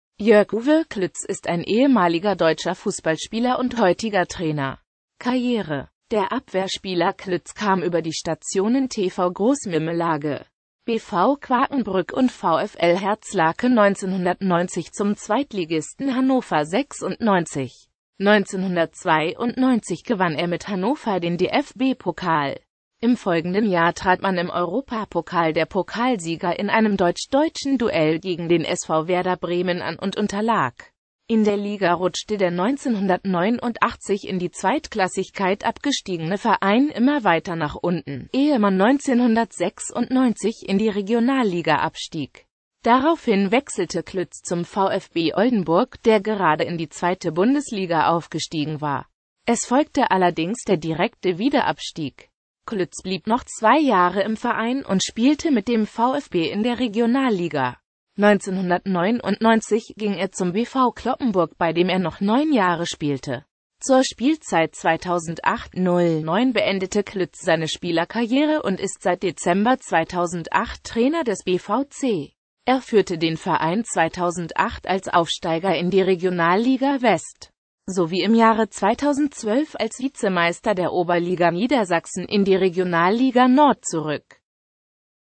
Jörg-Uwe Klütz
Vielen Dank für Ihre Unterstützung:

Jörg-Uwe Klütz (* 21.Juli 1968 in Quakenbrück) ist ein ehemaliger deutscher Fußballspieler und heutiger -trainer.